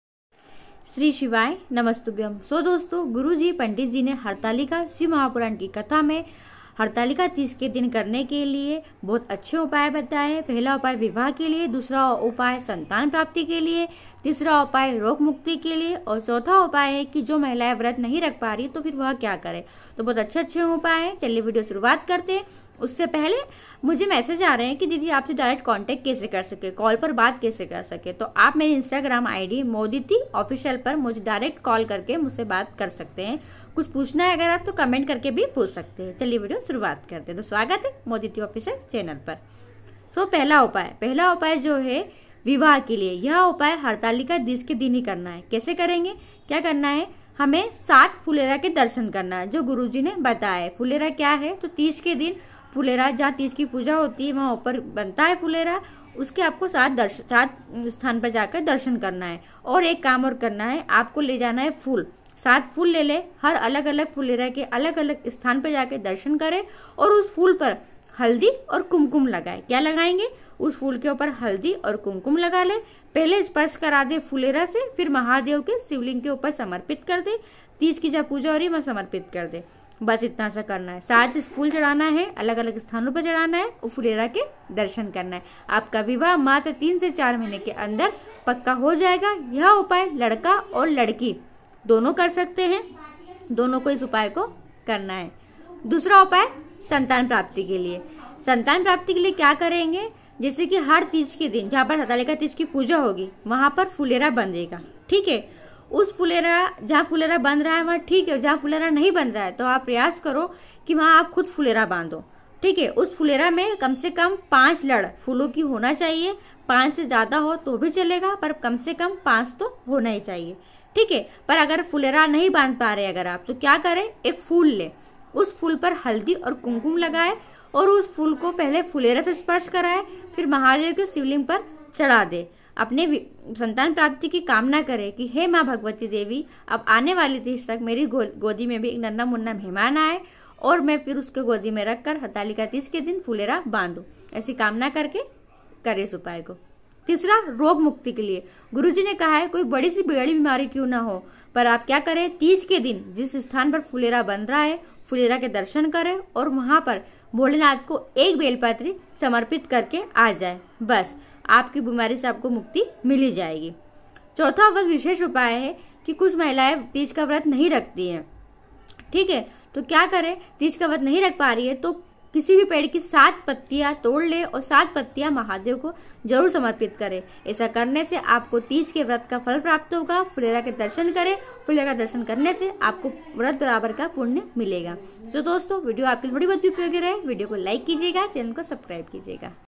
30अगस्त हरतालिकातीज के दिन करे उपाय विवाह ,संतान प्राप्ति ,रोग मुक्ति और जो व्रत नहीं रखता उनके लिए
30अगस्त हरतालिकातीज के दिन करे उपाय विवाह ,संतान प्राप्ति ,रोग मुक्ति और जो व्रत नहीं रखता उनके लिए